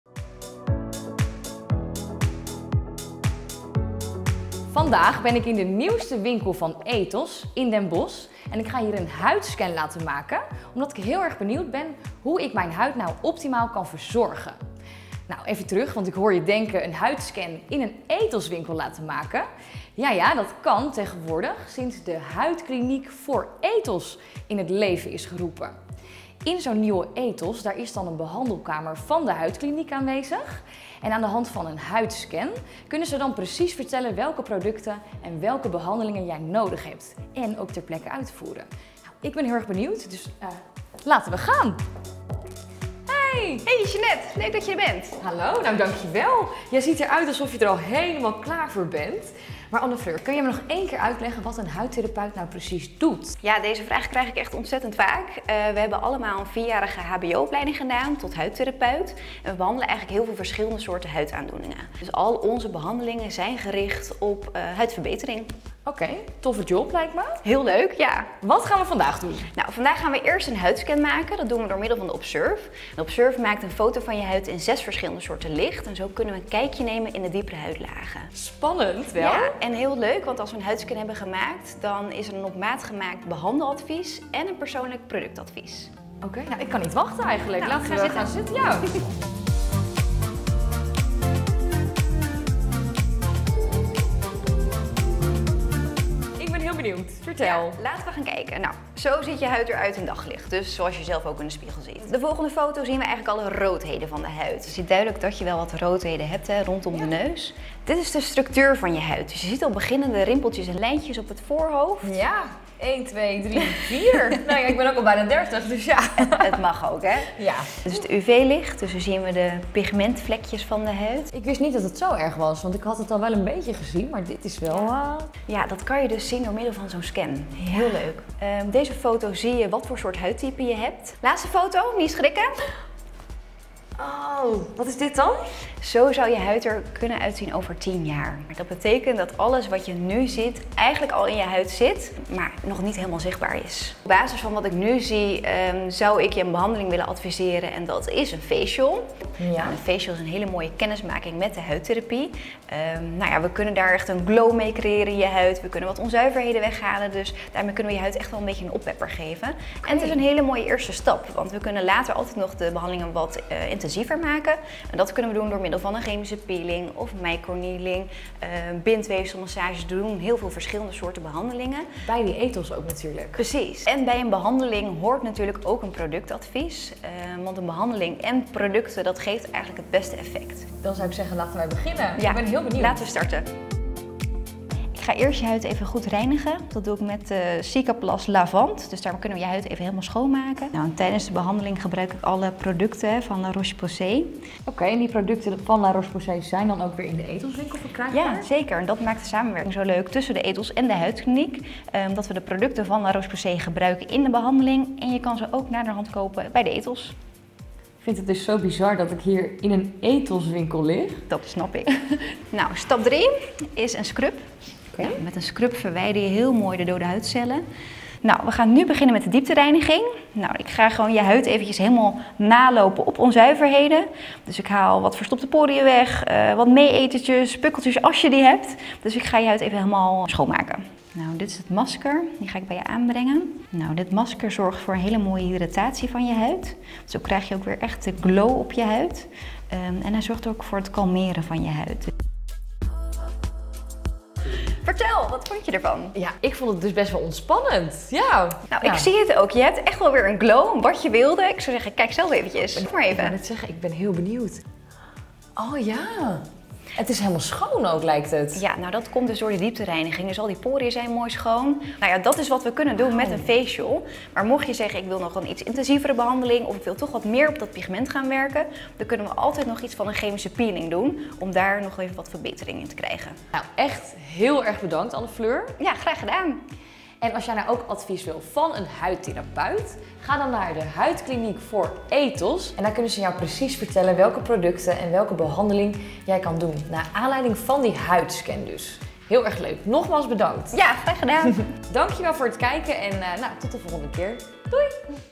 De Huidkliniek voor Etos
Met veel trots lanceren wij vandaag onze eerste kliniek samen met Etos in Den Bosch! Maak een huidscan, krijg productadvies en onderga een huidbehandeling in een van de 2 behandelkamers. Zien we je snel?🧡